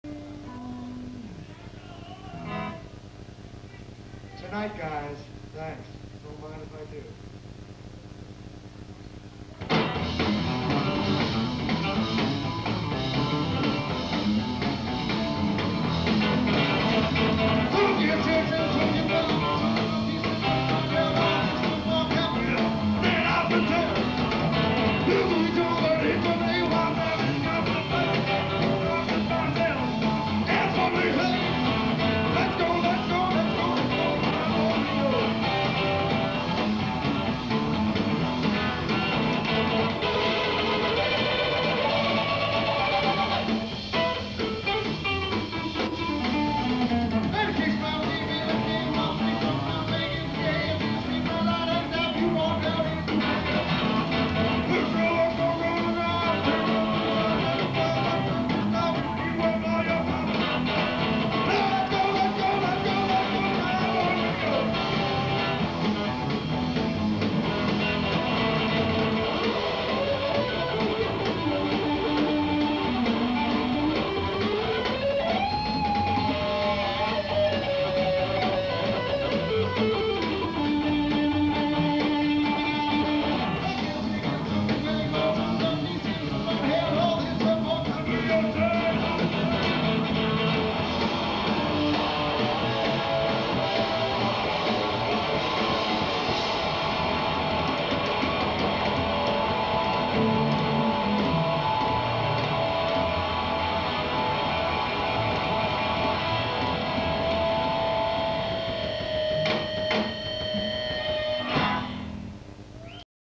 Tom Troccoli's Dog featuring Greg Ginn on bass Dec. 20 1984
Filmed at the Embassy Hotel.
Opening for Black Flag.

Soundboard audio feed.
Filmed with the band's permission.